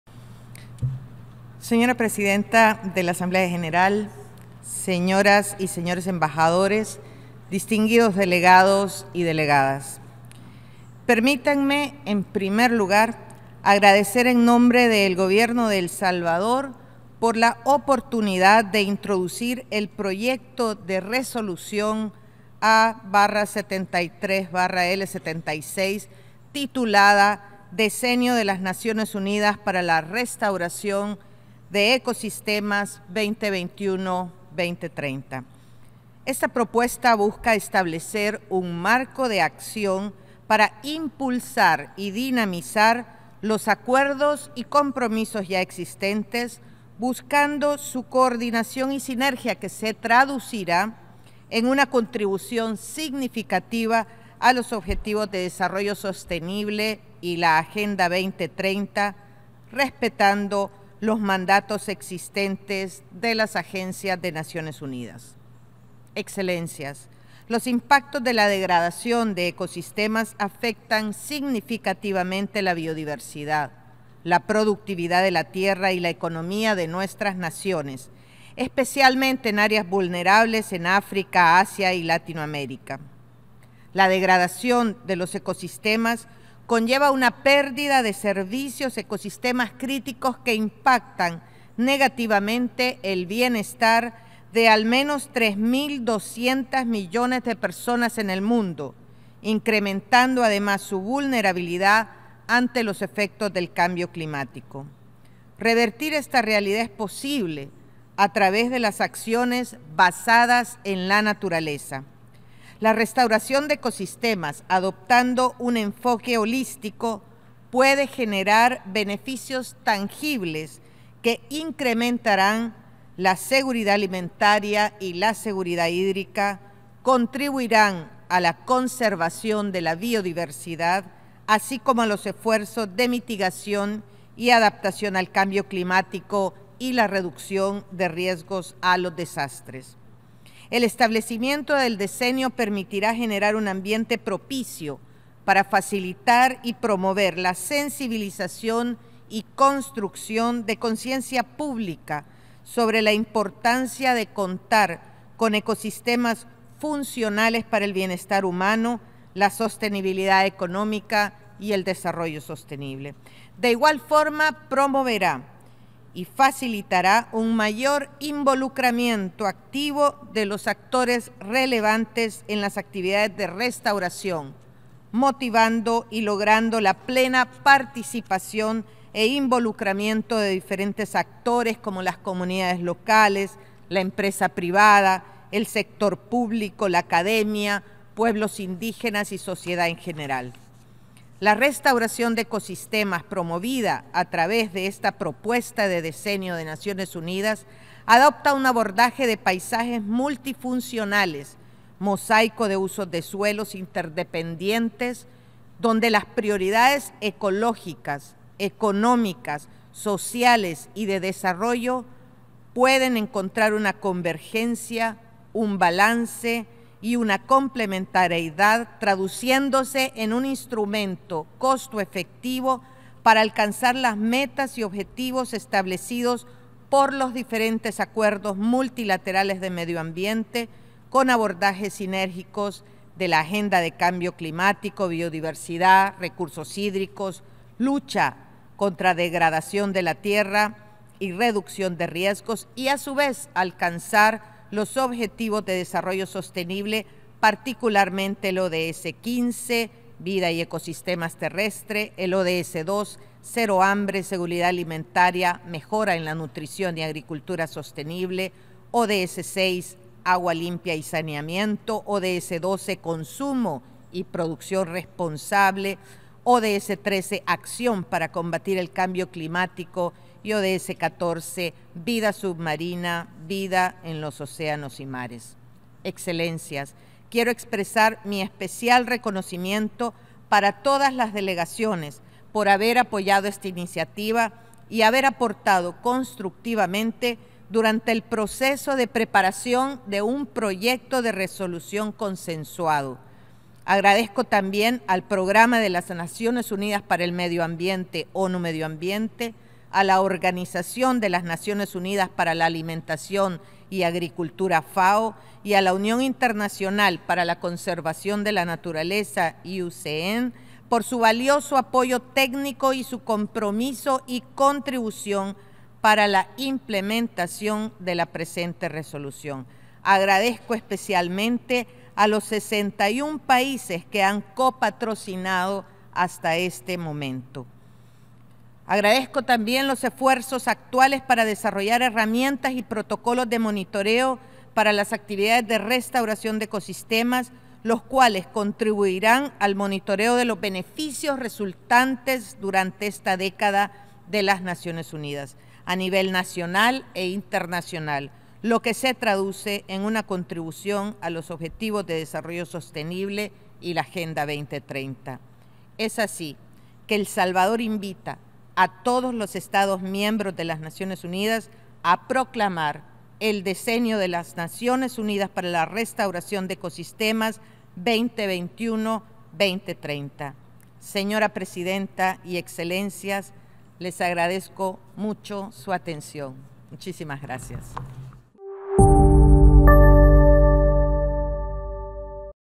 Discurso ministra Pohl por década de la Restauración de los Ecosistemas (2021-2030)
En un hecho histórico, el Ministerio de Medio Ambiente y Recursos Naturales (MARN) puso a El Salvador en el mapa mundial. Hoy se adoptó por consenso la “Década de las Naciones Unidas para la Restauración de Ecosistemas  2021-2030”, una iniciativa impulsada por El Salvador.